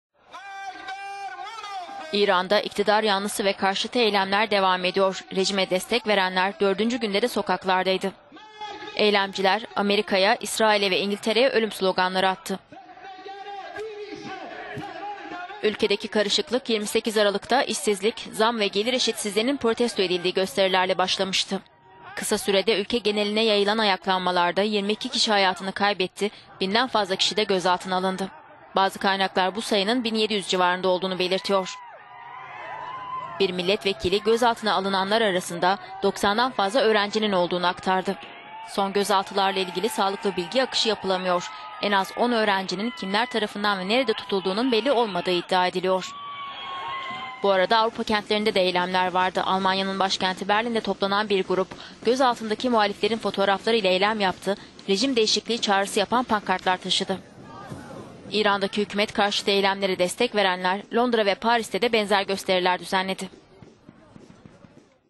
"İran'da en az 90 öğrenci gözaltında"
İran'da en az 10 öğrencinin kimler tarafından ve nerede tutulduğunun belli olmadığı iddia edildi